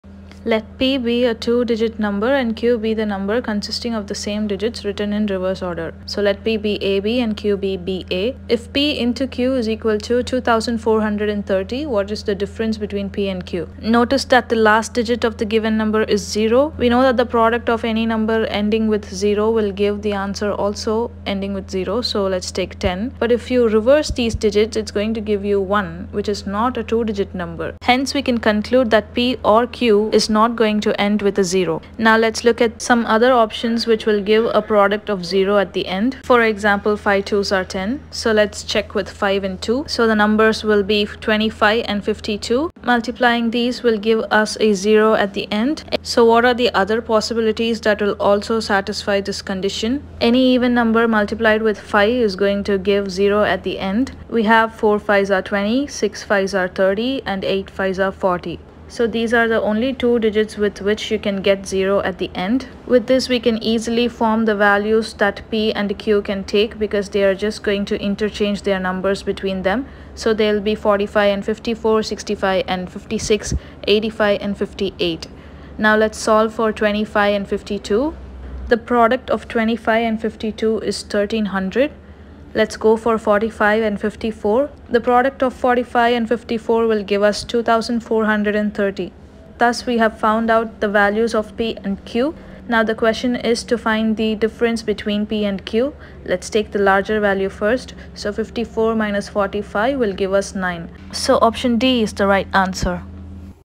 UPSC CSE Prelims - CSAT 2022 Q.66)Let p be a ...
Q.66)Let p be a two-digit number and q be the number consisting of same digits written in reverse order. If p × q = 2430, then what is the difference between p and q?